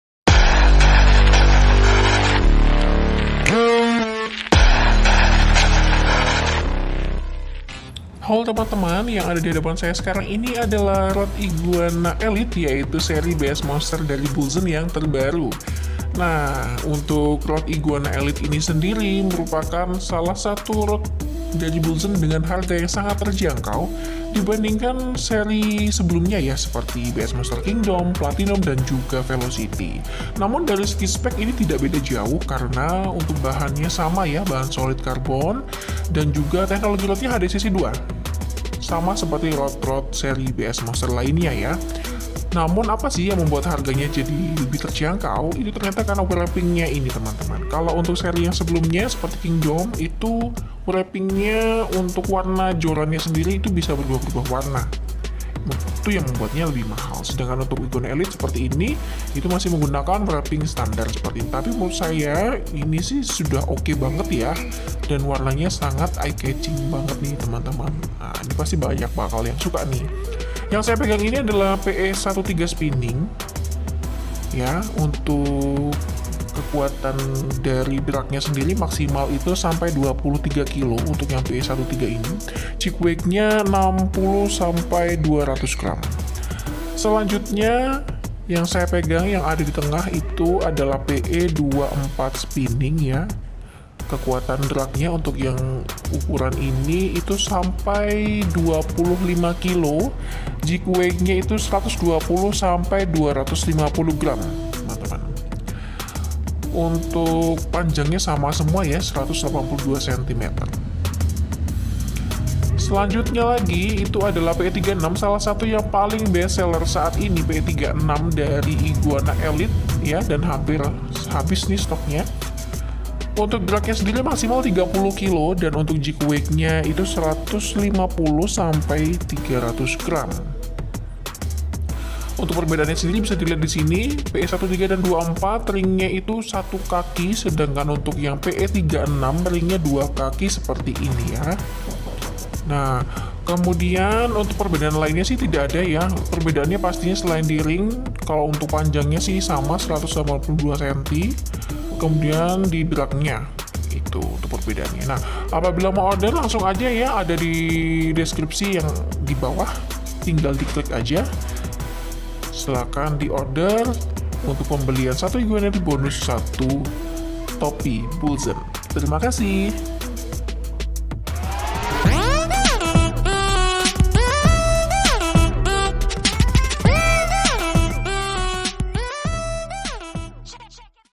Rod Bullzen IGUANA ELITE | Bullzen Fishing Indonesia
" FREE / GRATIS TOPI BULLZEN !

JORAN BULLZEN BS MONSTER IGUANA ELITE

→ Bullzen Original Berhologram
→ Harga Sudah Termasuk Packing Pipa
→ One Piece ( Tanpa Sambungan )
→ Bahan Solid Carbon HDCC2
→ Panjang 182cm
→ Ring Guide Merk Bullzen
→ Reel Seat Merk Bullzen

Ready Varian :

♦ PE 1-3 Spinning  : Jig Weight 80-200 Gram l Max Drag 23 Kg

♦ PE 2-4 Spinning : Jig Weight 120-250 Gram l Max Drag 25 Kg

♦ PE 3-6 Spinning  : Jig Weight 150-300 Gram l Max Drag 30 Kg